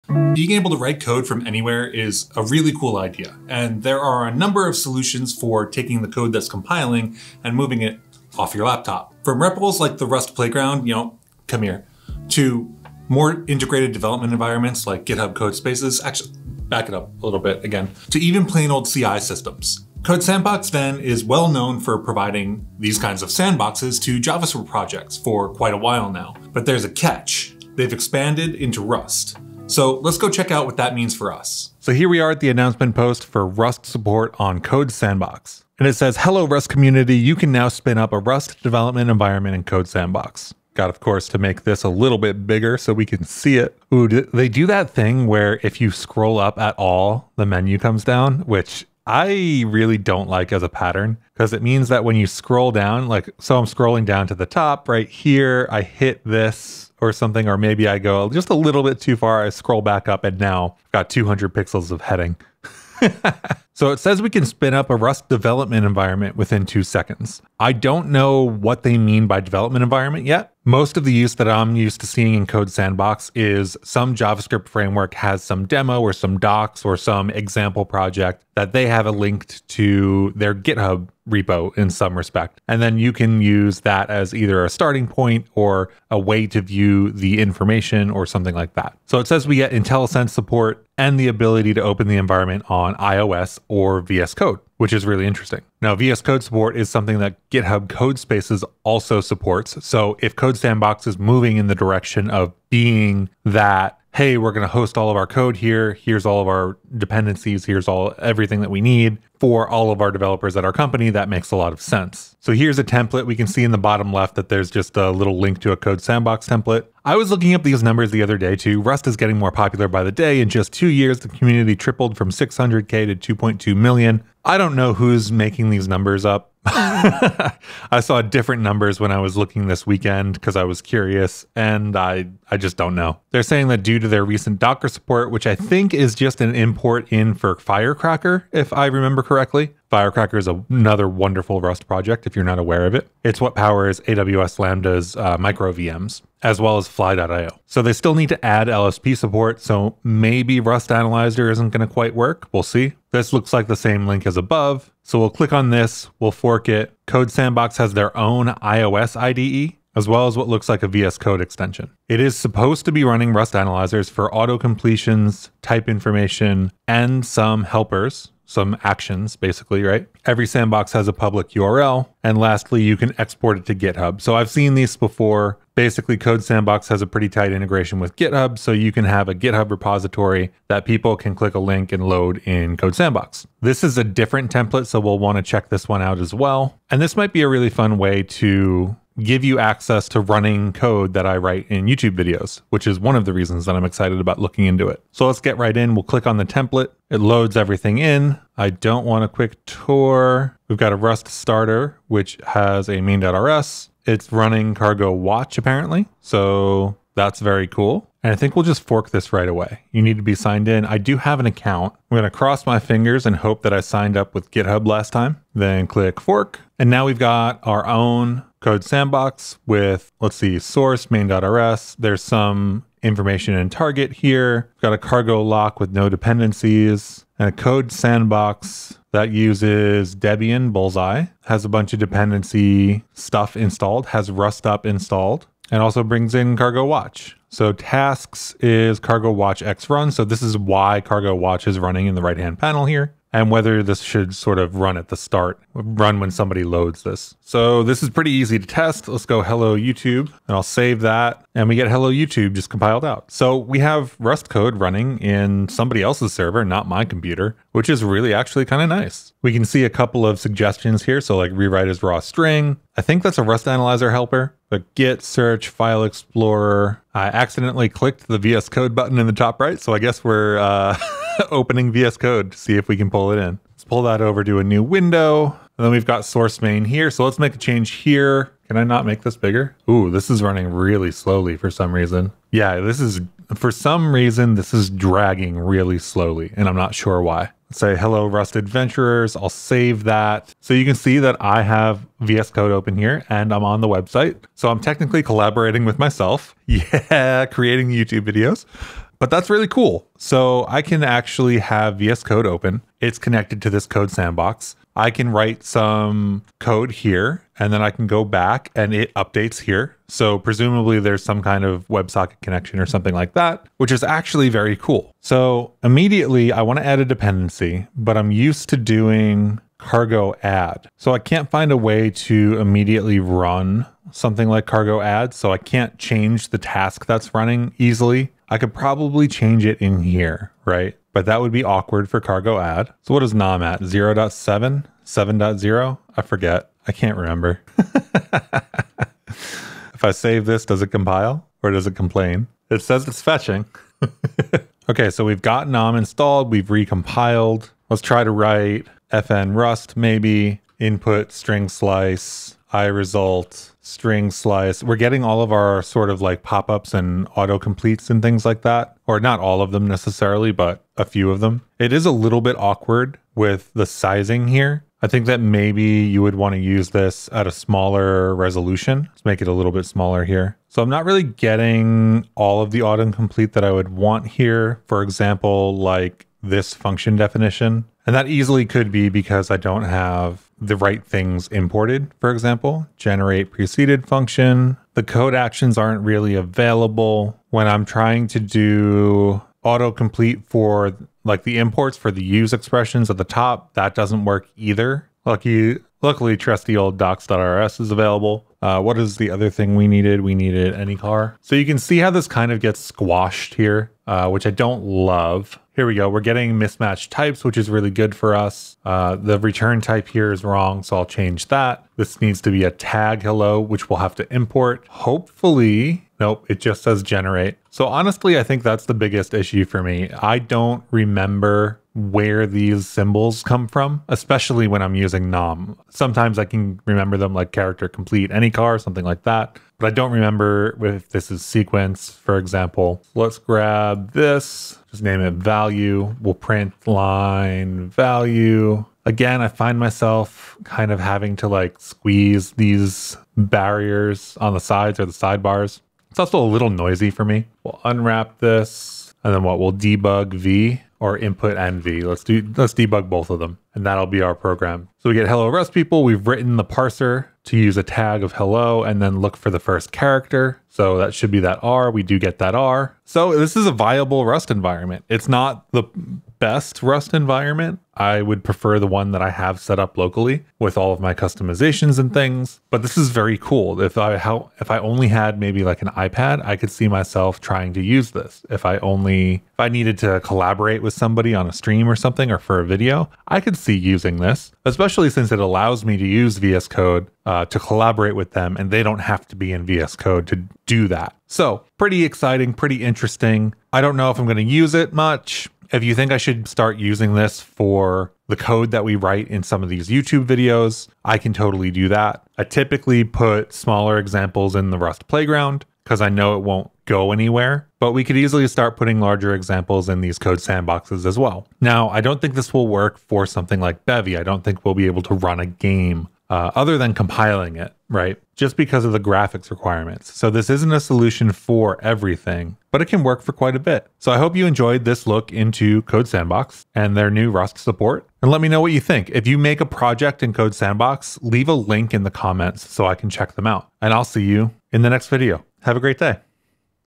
Compile Rust without your computer on CodeSandbox
You can now spin up a Rust development environment in CodeSandbox.

This update allows you to spin up a Rust development environment within 2
seconds, with IntelliSense support and the ability to open the environment in iOS or VS Code.